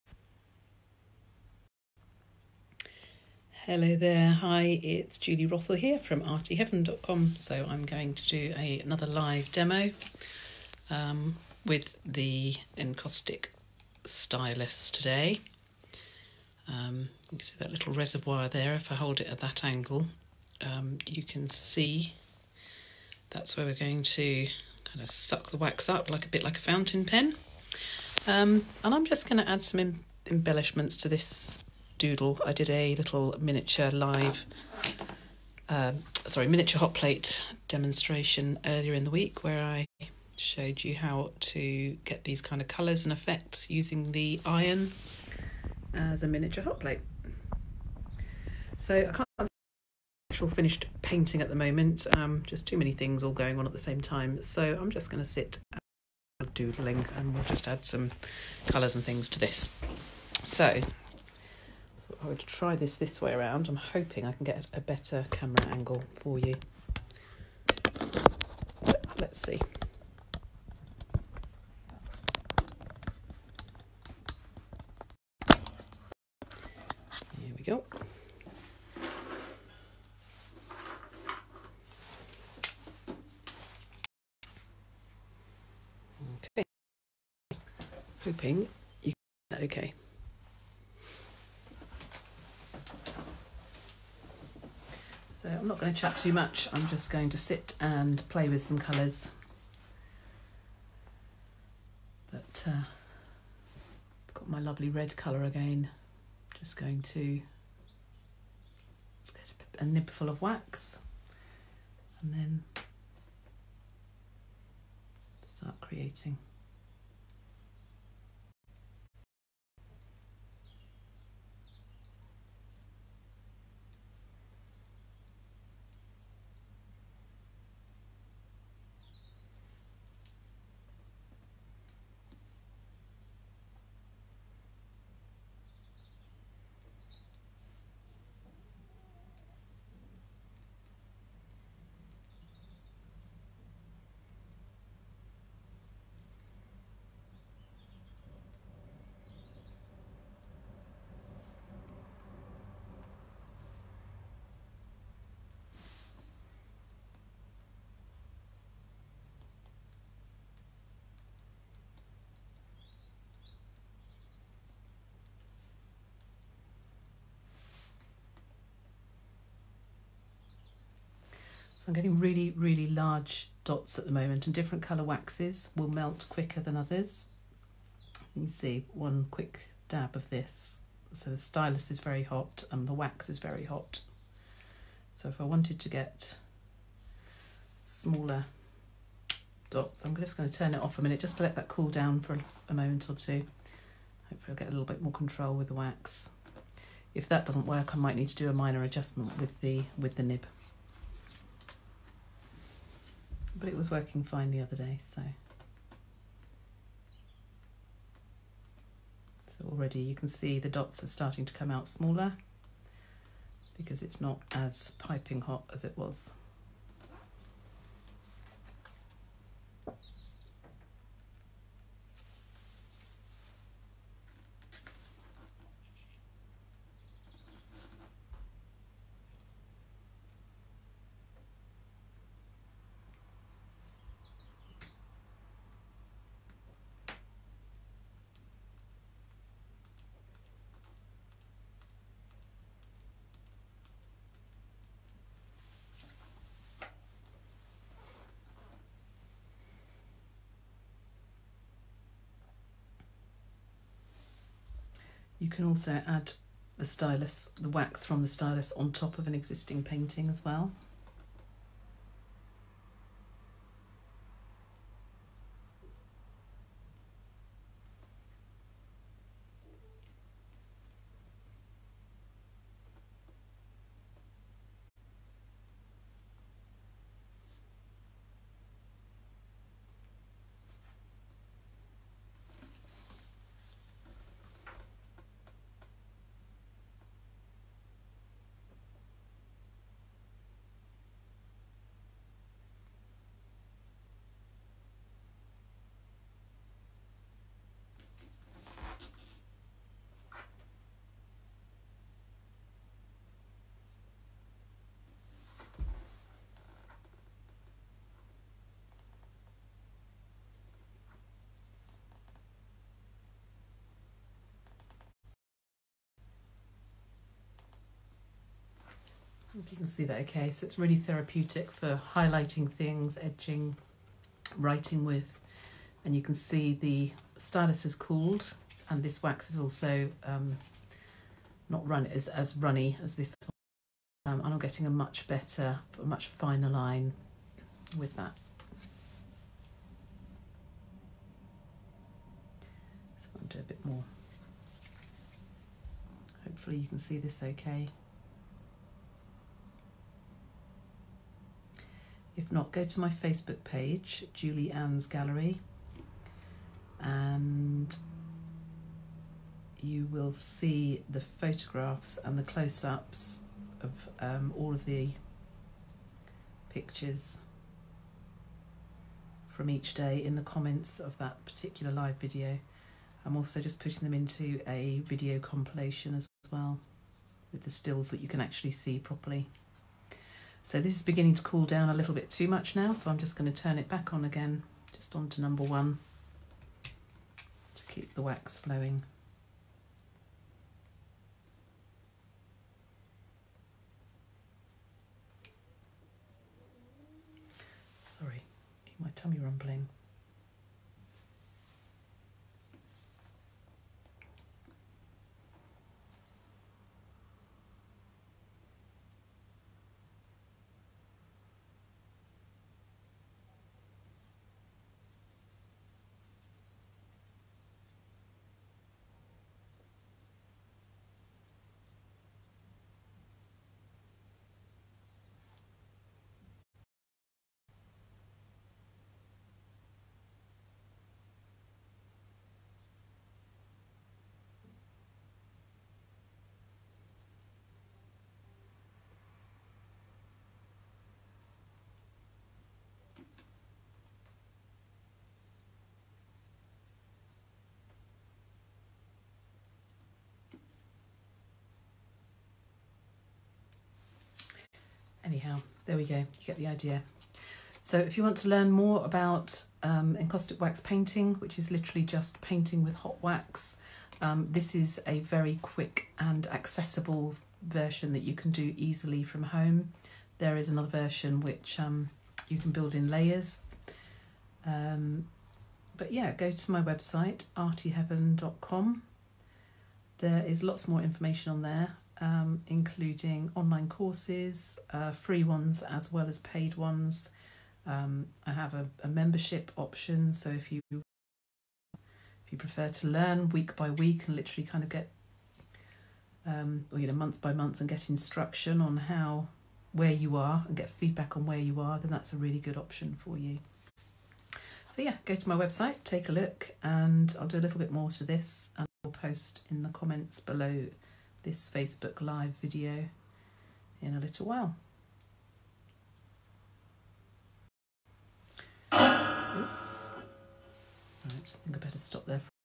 Doodling with the encaustic stylus...
Just going to doodle today, with the stylus...

I can’t seem to get my head on a finished picture this week... but doodling is still good too!

Get that wax flowing... I’m going to add some embellishments to one of the mini hotplate demos I did earlier in the week. 😊🐝🎨✨